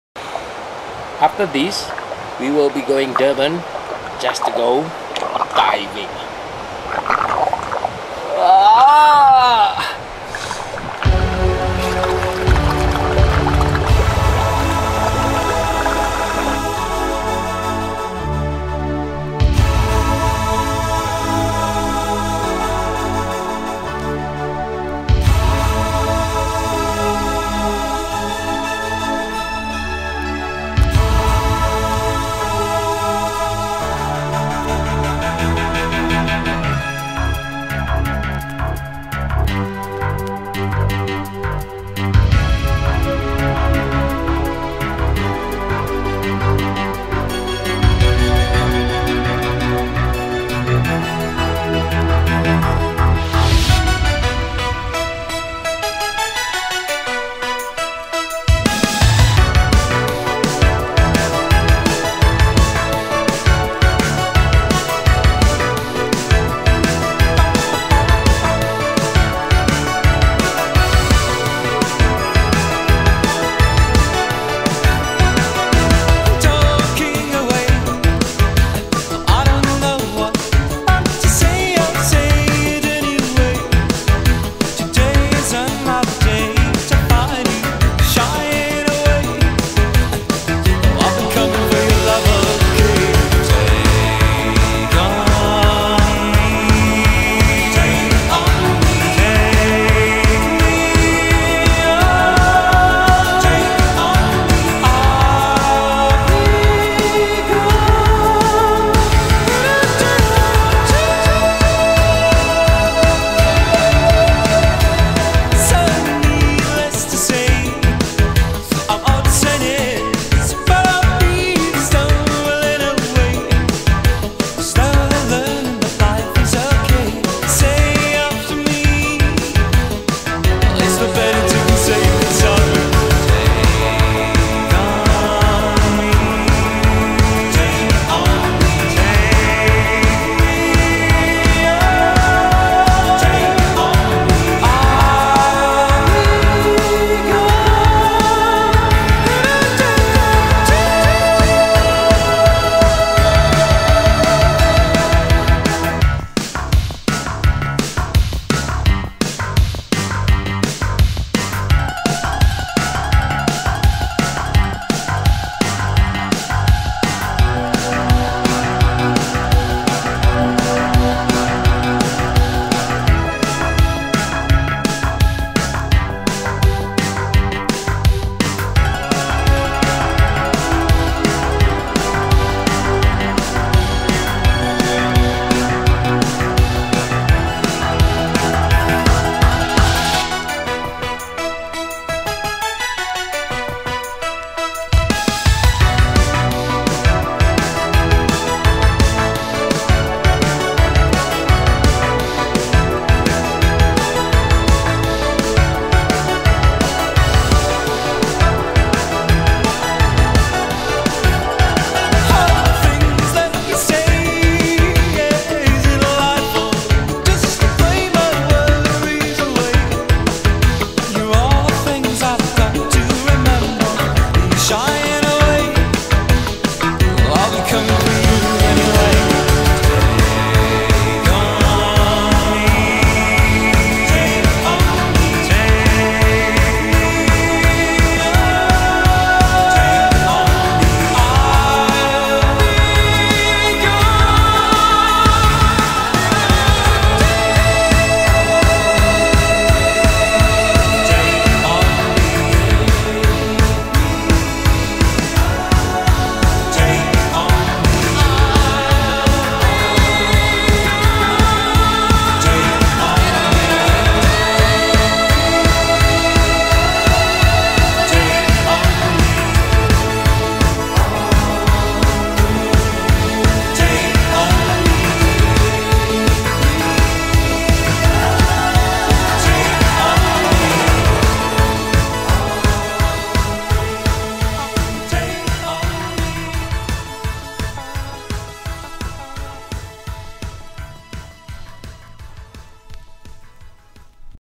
Durban shark diving 2011.wmv
after a week of safari in kenya we head down to Durban to enjoy a week of shark diving, it was something that you have to try at least once a life time : )